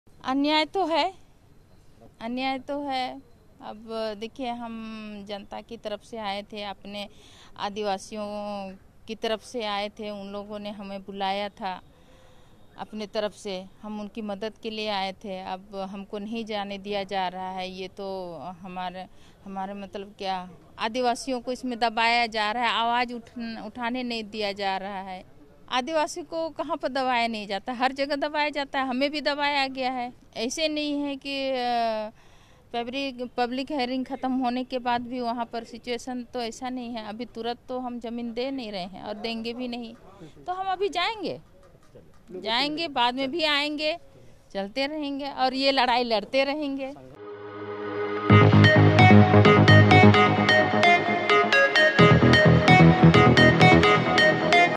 ‘ଆଦିବାସୀଙ୍କୁ ଦବେଇ ଦିଆଯାଉଛି’ | Odisha Reporter
ଝାଡଖଣ୍ଡ ମୁକ୍ତିମୋର୍ଚ୍ଚାର ଓଡିଶା ରାଜ୍ୟ ସଭାପତି ଅଞ୍ଜଳି ସୋରେନ ଓ ତାଙ୍କ ସହଯୋଗିଙ୍କୁ ଗତ ତିନଦିନ ହେଲା ଏକ ହୋଟେଲରେ ପୁଲିସ୍  ନଜର ବନ୍ଦି ରଖିଥିବା ଅଭିଯୋଗ ହୋଇଥିଲା । ରାଉରକେଲା ପୁଲିସ୍ ସେମାନଙ୍କୁ ହୋଟେଲରୁ ବାହାରକୁ ଯିବାକୁ ଅନୁମତି ଦେଉନ ଥିଲା । ତେବେ ଆଜି ଜେଏମଏମ ଓ ପୁଲିସ୍  ପ୍ରଶାସନ ସହ ଆଲୋଚନା ପରେ ଏକ ପ୍ରତିନିଧି ଦଳ ରାଉରକେଲା ଏଡିଏମଙ୍କୁ ଭେଟି ଏକ ଦାବି ପତ୍ର ଦେଇଛନ୍ତି । ଜେଏମଏମ ଦାବି କରିଛି ଆଦିବାସୀଙ୍କ ସ୍ୱରକୁ ସରକାର ଚପାଇ ଦେବାକୁ ଉଦ୍ୟମ କରୁଛନ୍ତି ।